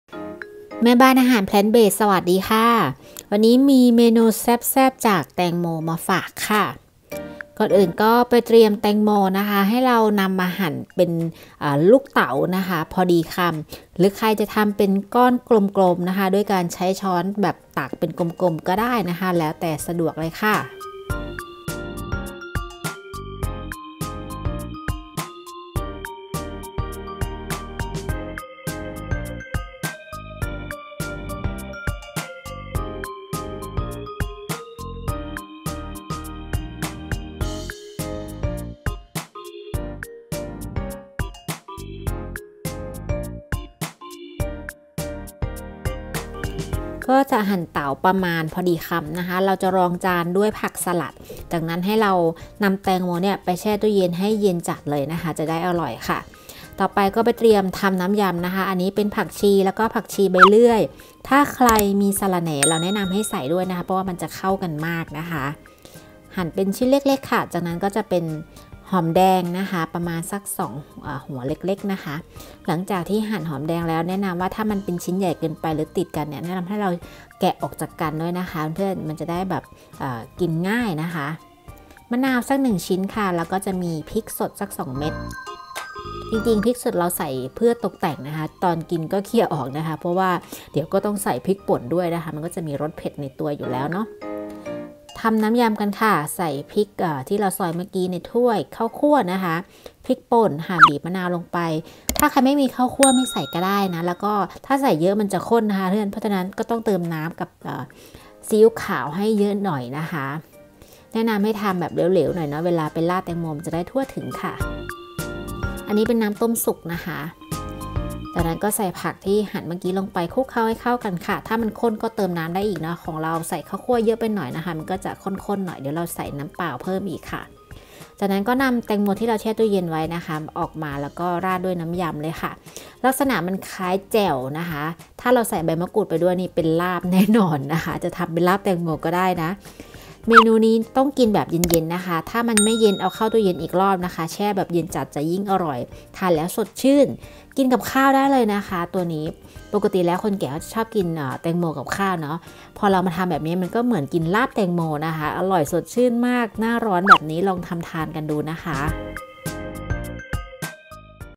ยำแตงโม Watermelon Salad | Vegan
ส่วนผสม ingredients
แตงโม__watermelon
สะระแหน่__mint
ผักชีฝรั่ง__long coriander
ซีอิ๊วขาว__soy sauce
น้ำมะนาว__lime juice
พริกป่น__chili flake
พริกสด__chili
ข้าวคั่ว__roasted rice